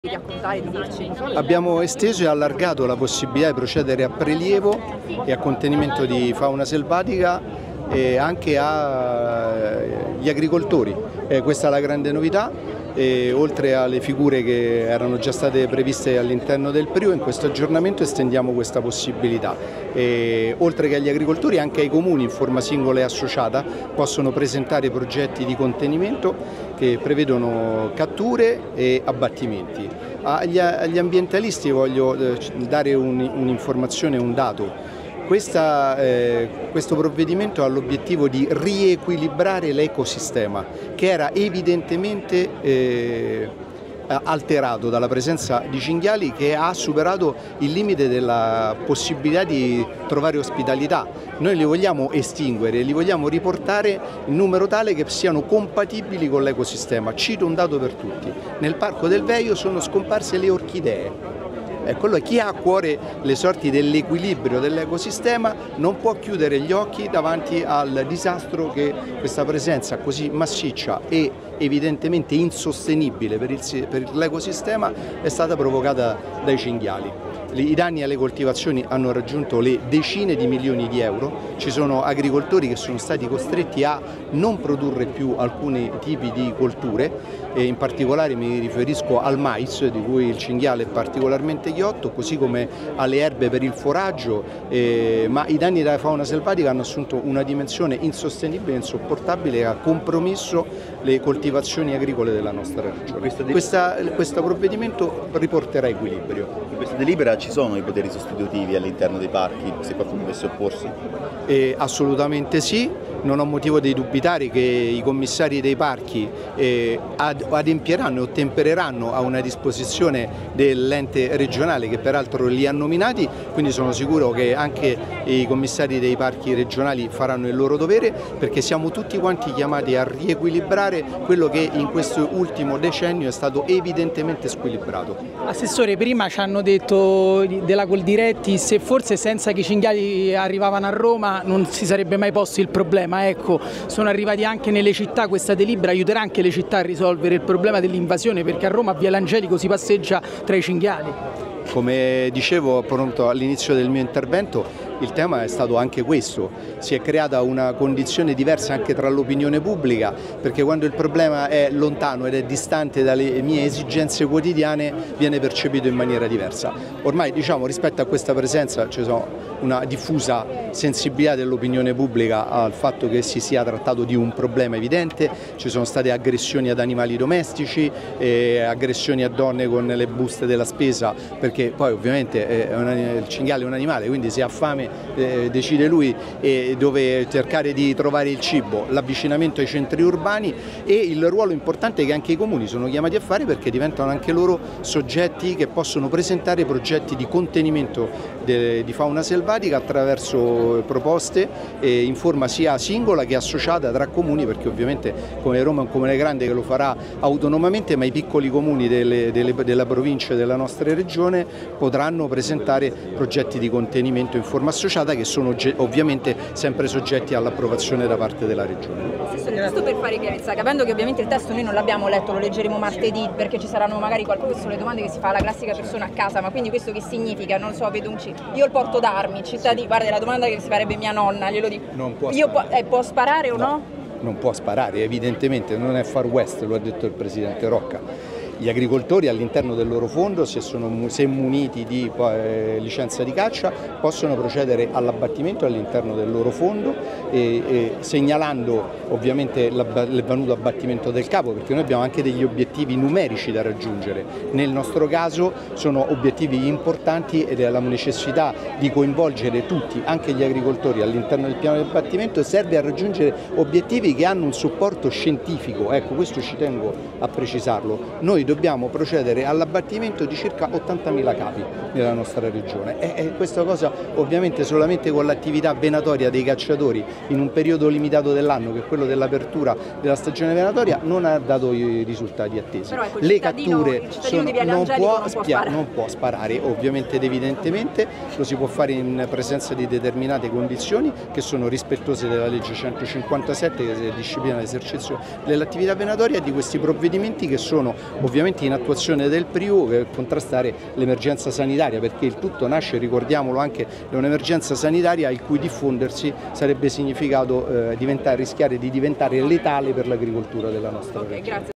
Delibera abbattimento cinghiali Lazio, Righini: “Comuni potranno proporre piani, non sarà Far West”
L’assessore della Regione Lazio Giancarlo Righini sottolinea come nelle città  in ci sarà alcun far west e le persone persone non potranno sparare liberamente contro i cinghiali.